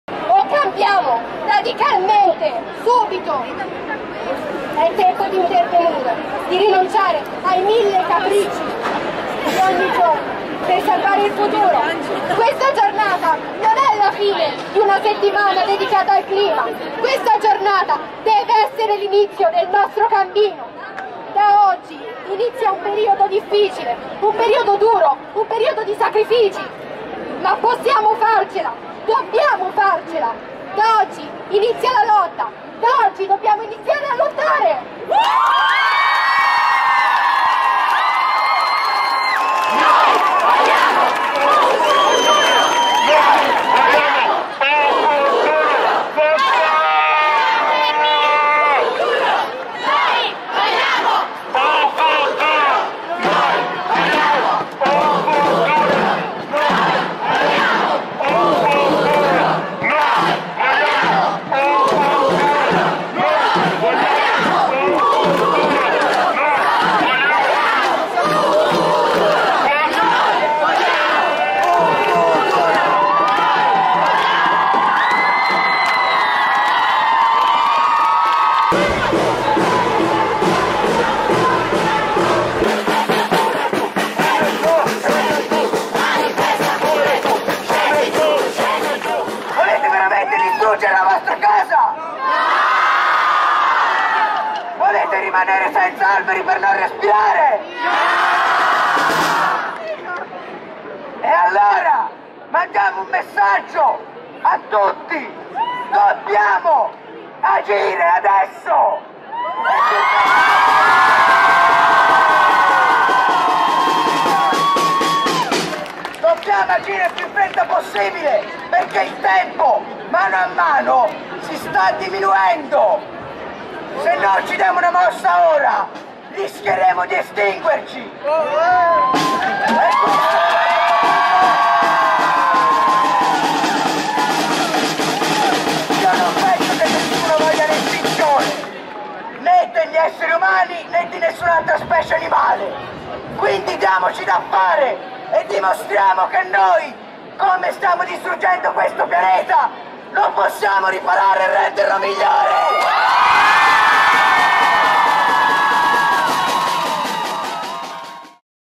Sciopero del clima, in migliaia in piazza a Pistoia
Striscioni, cartelli, slogan per la manifestazione mondiale per chiedere il cambiamento del clima (Video Lorenzo Gori)